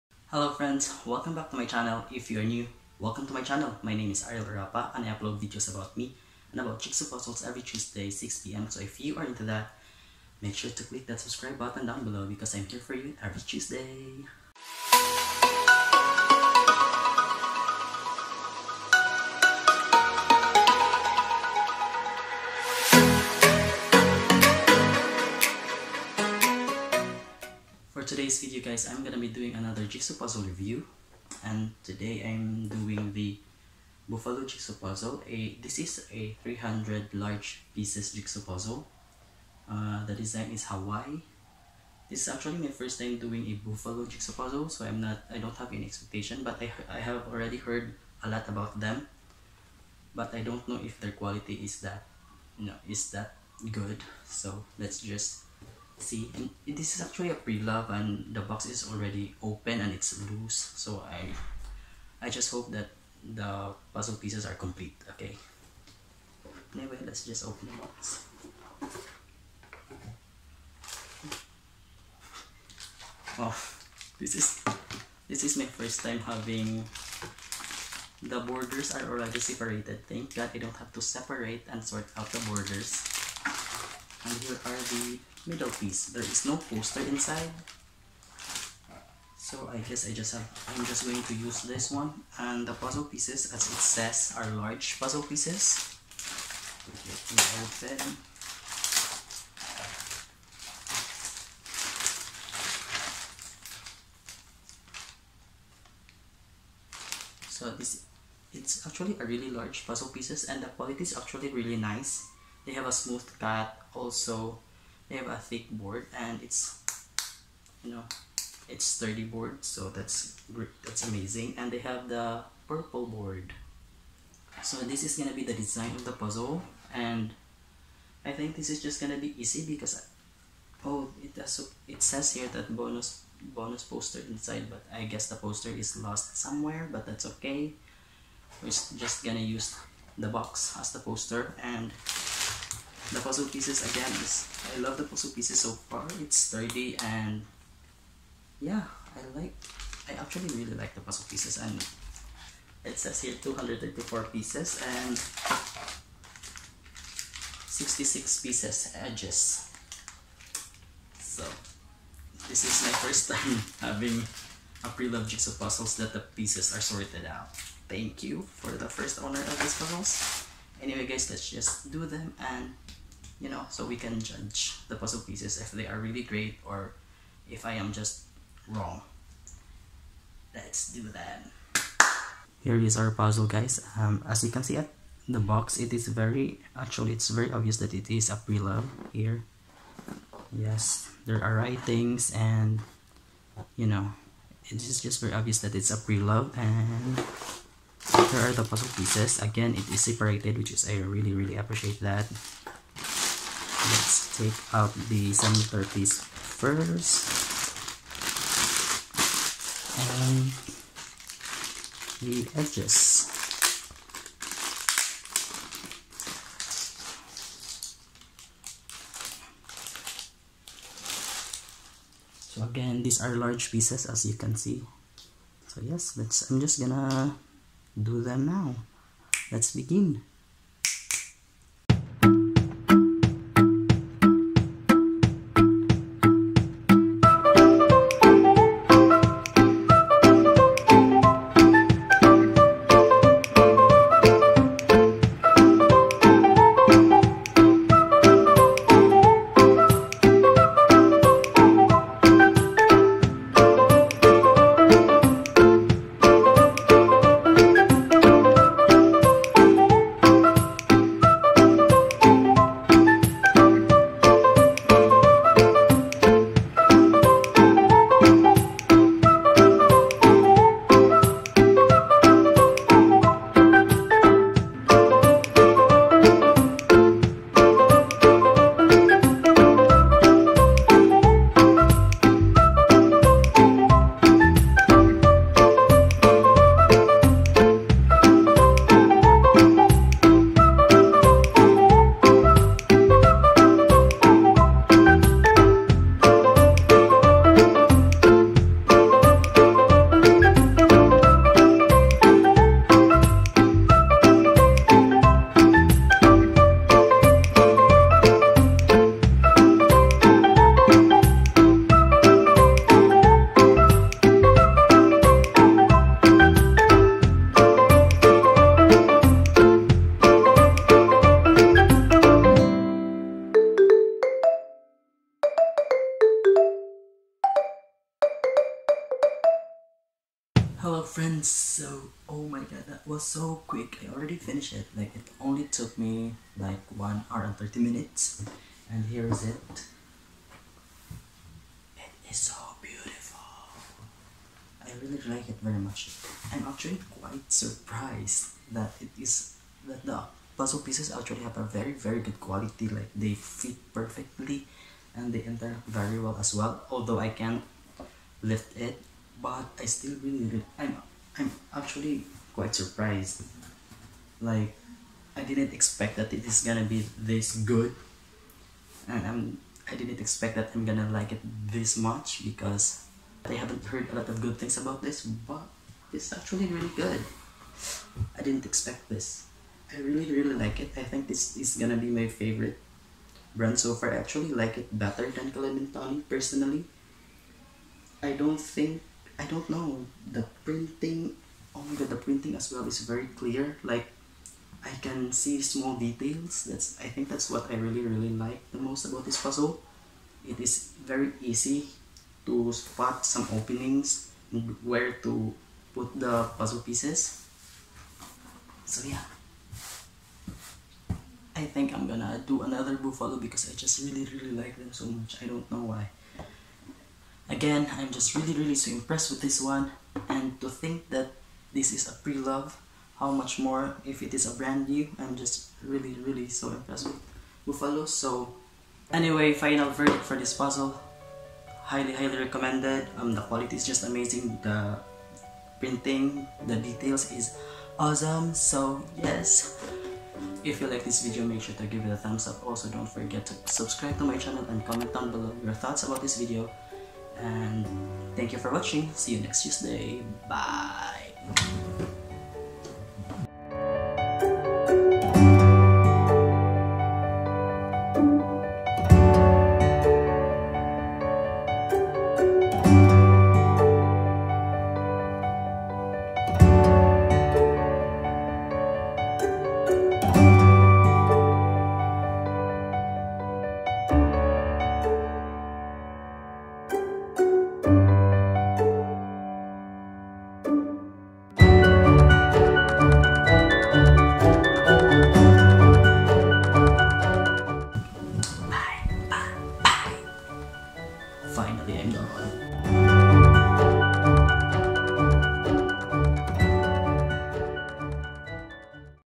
Buffalo Jigsaw Puzzle Review | My First Large Pieces Jigsaw Puzzle
Easiest Puzzle so far!

Hello, friends! It's Tuesday again.
Here are my thoughts on Buffalo Large Pieces Jigsaw Puzzle. This is my first time doing a Buffalo and doing large pieces as well. I've done tiny puzzles but have not the done the large ones. So yes, I'm quite excited about this one.

Puzzle Details:
Buffalo
Hawaii
300 Large Pieces

I bought this puzzle at Workingmoms Mktg, you should definitely check them out in this link:

Bye, be well!